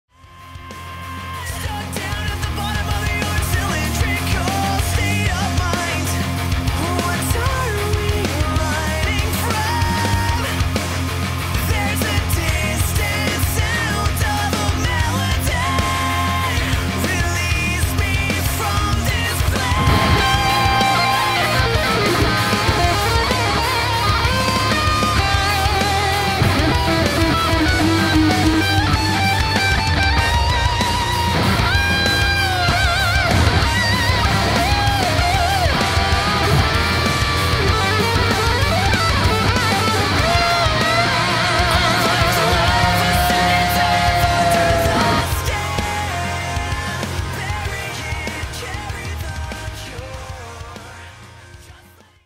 toontrack metal guitar god
This is my entry for the Metal God 2013 competition